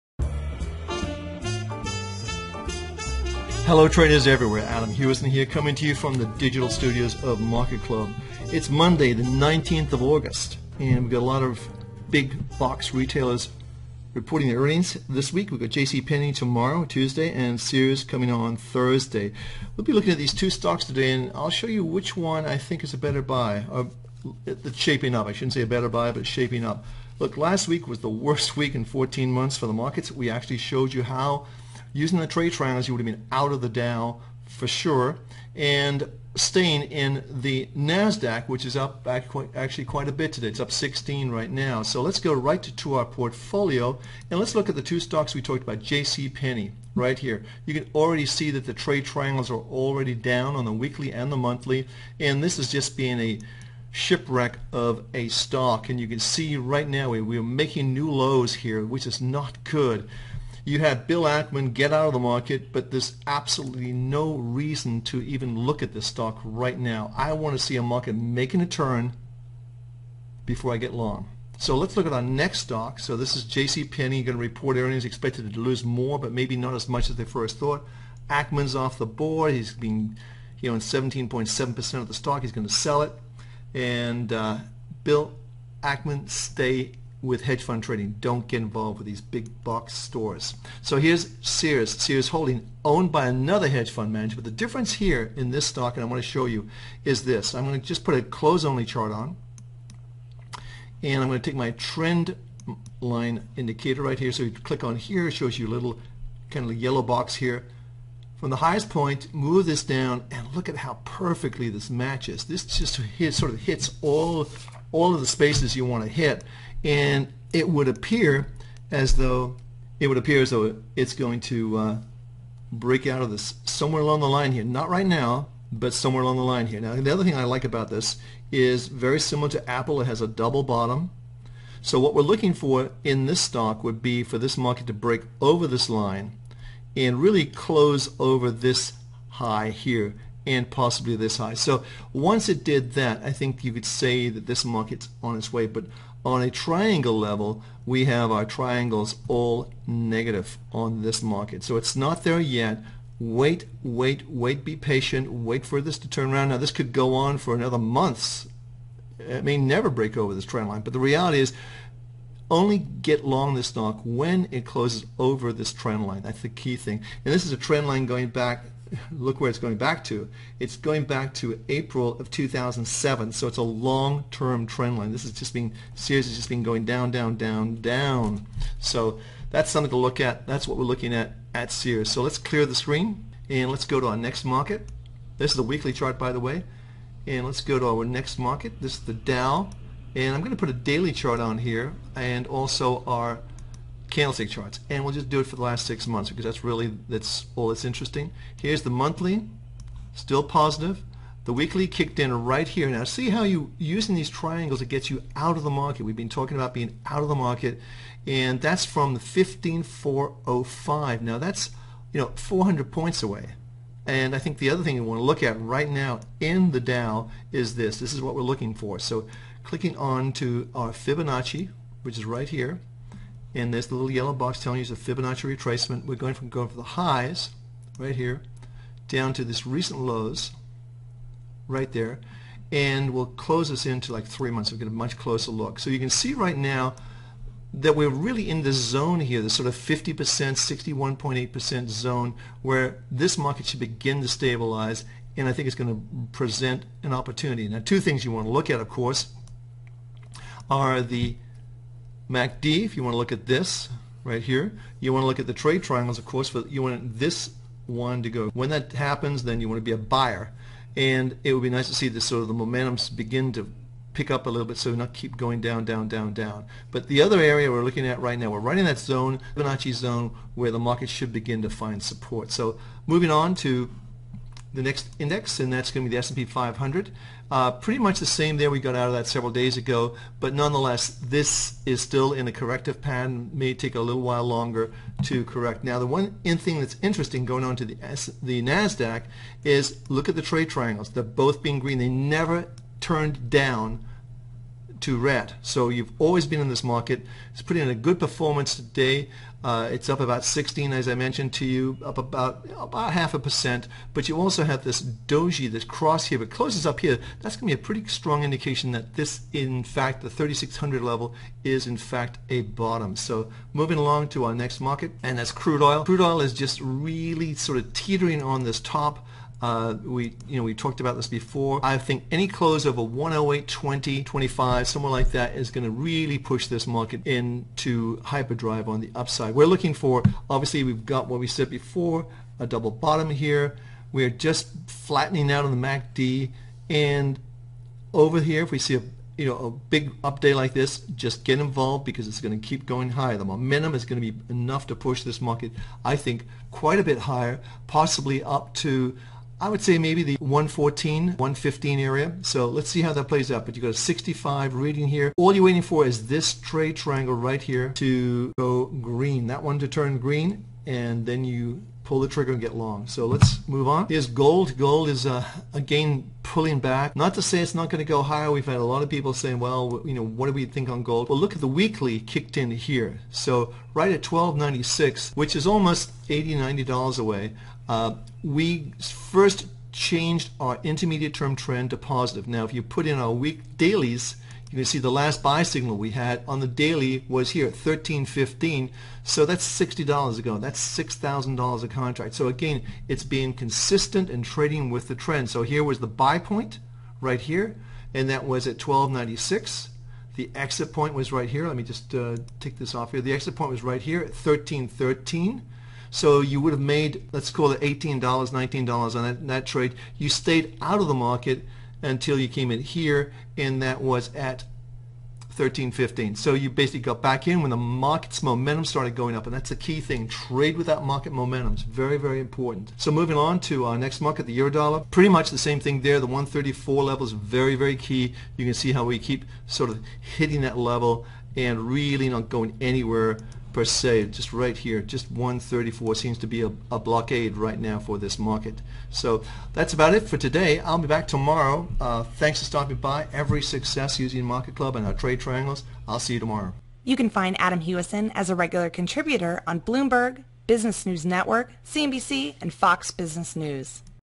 J.C. Penney & Sears report earnings this week...guess which one I'm interested in?
Hello traders everywhere! Adam Hewison here, President of INO.com and Co-creator of MarketClub, with your mid-day market update for Monday, the 16th of August.

The meltdown last week, the worst in 14 month for the markets, was not a good one for most investors, but it was not unexpected based on our Trade Triangle technology. This week, it's a whole new picture for the markets as we have earnings coming out on some big box retailers, JC Penney (NYSE:JCP) on Tuesday and Sears (NASDAQ:SHLD) on Thursday.

There have been a lot of acrimonious events surrounding JC Penney. Bill Ackman, who just resigned his board seat last Friday, now has an agreement in place with the company to unload his shares over time. Mr. Ackman owns 17.7% of JC Penney and is its largest shareholder. One can only guess how much money he has lost in this ongoing shipwreck of a stock. Finally, he's woken up and realized that he's a hedge fund manager, not a manager of a big box retail store.

Next comes Sears, who reports earning on Thursday and is run by another hedge fund manager, Eddie Lampert. Why would these two hedge fund managers want to get into and manage old fashioned big box retail environments in a digital world? It is beyond me, but there is something that is interesting in one of these stocks that I want to share with you today. See if you agree with my analysis.

As always, I will be looking at all of the markets and bringing you my latest thoughts on what I see and what the Trade Triangles are showing us.